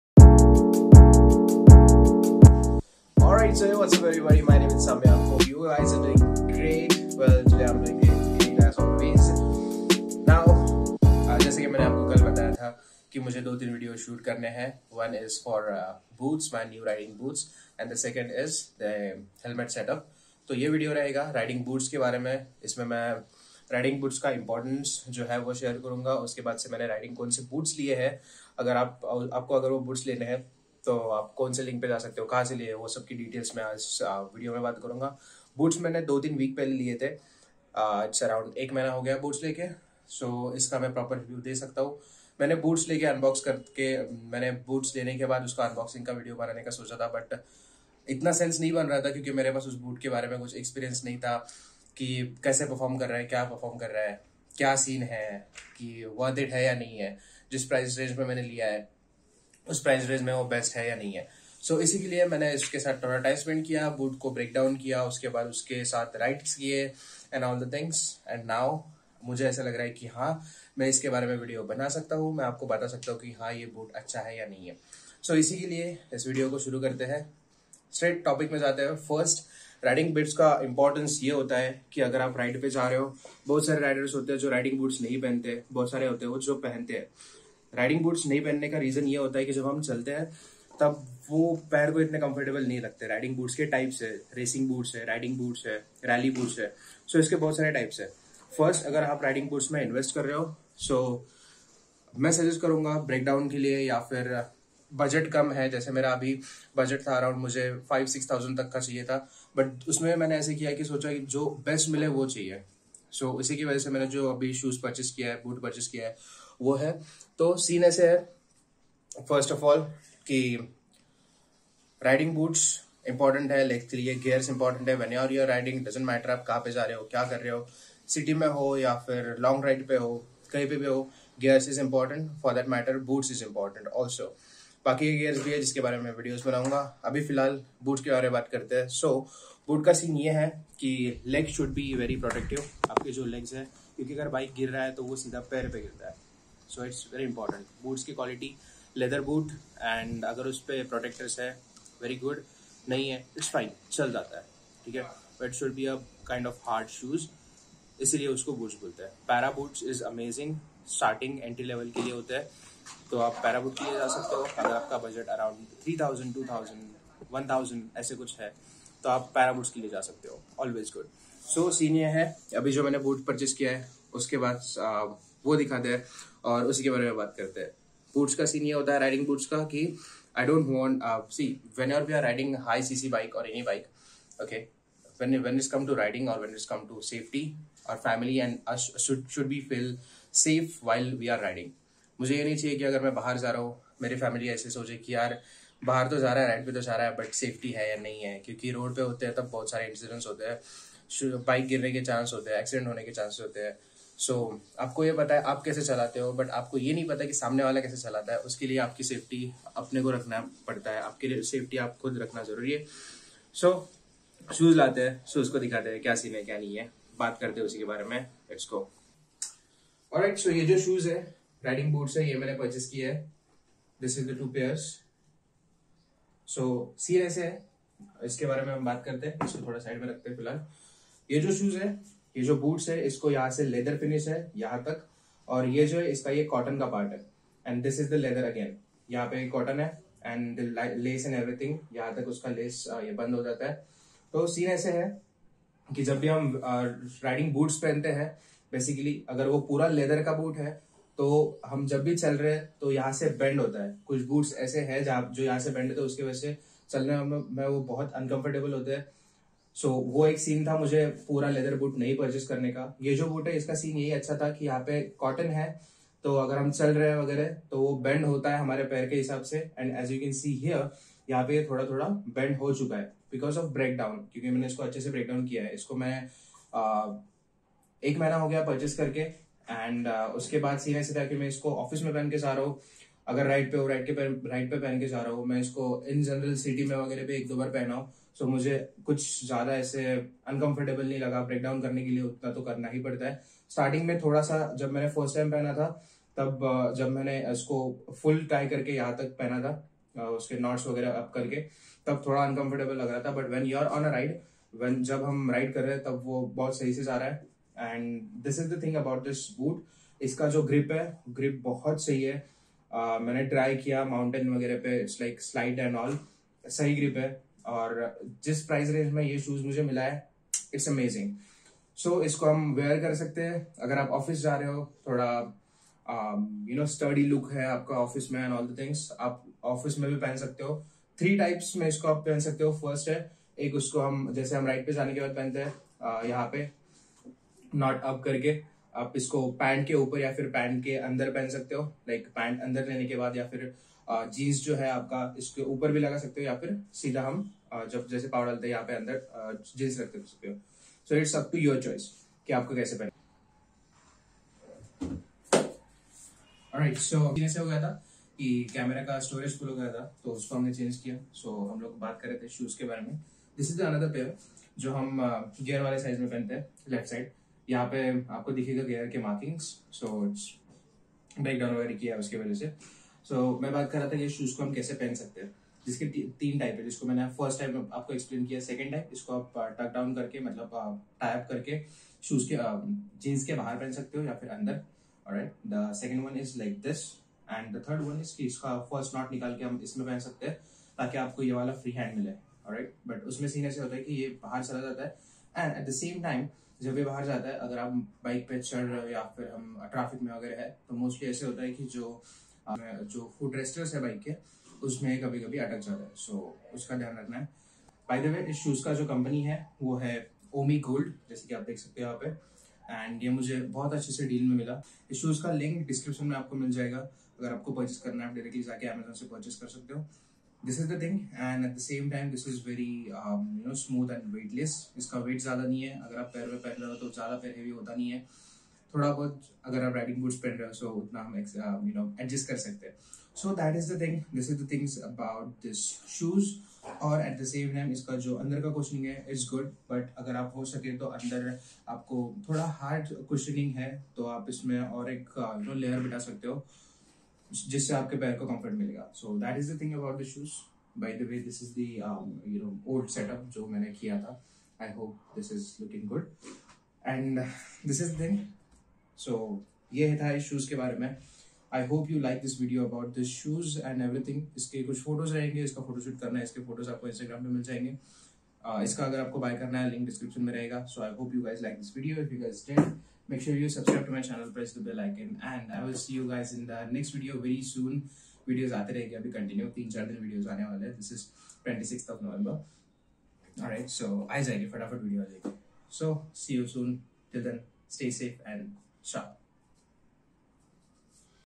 Best Riding Boot Under ₹1500 | Riding Boots Importance & Review | Samyak Ambhore | Pune
Hello Guys in this video I'm talking about riding boot that to under ₹3000. I hope you guys like this video.
Show link is here
OMEE GOLD Men's Black Leather Tactical Combat Ring NCC/Army/Military Boot